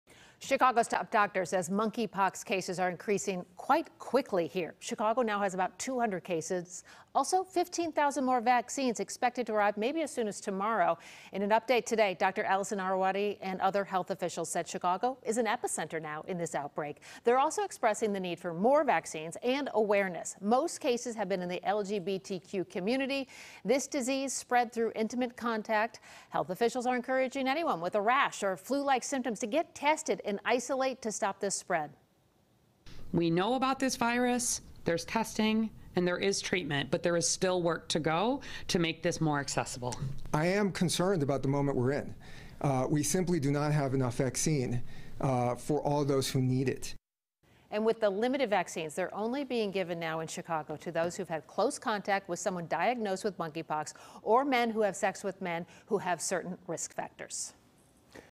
Monkeypox cases rising in Chicago, city's top doctor warns
Chicago's top doctor says monkeypox cases are increasing quite quickly in the city.